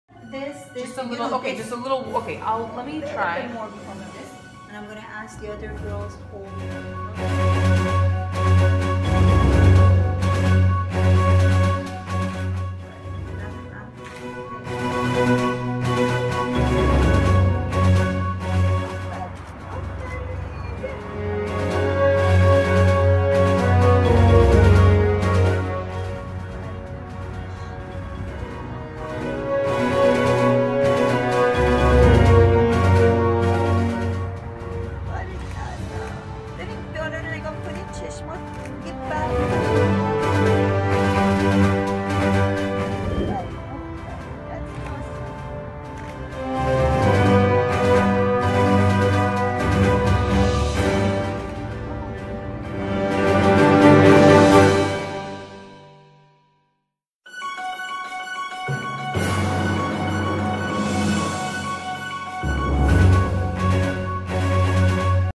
Homa Sarshar
Enjoying the Photoshoot of 50th anniversary, Homa & Nejaat Sarshar. Telling their story of being together for 50 years with all ups and downs through my camera lens and bring them as a fine hand crafted album to stay for generations as an evidence. G